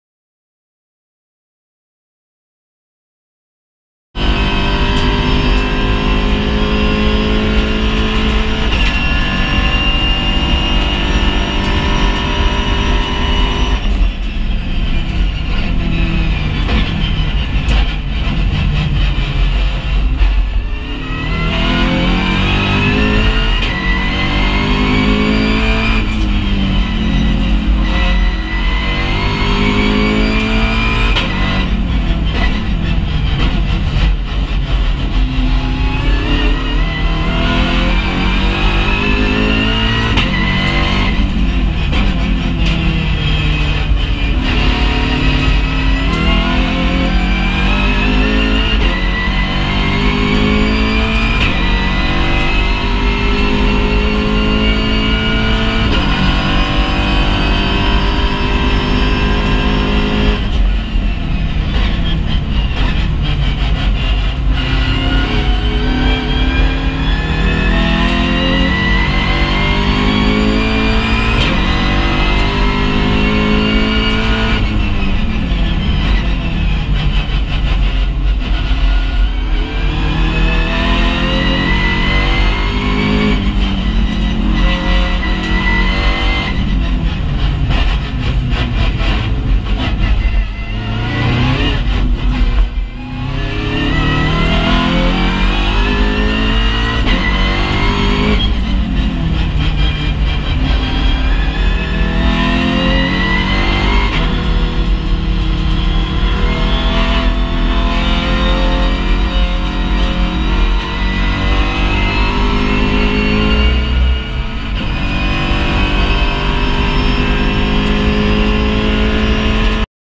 César Campaniço - VW Golf GTI TCR
Primeiros terstes com o novo VW Golf GTI TCR da equipa Team Novadriver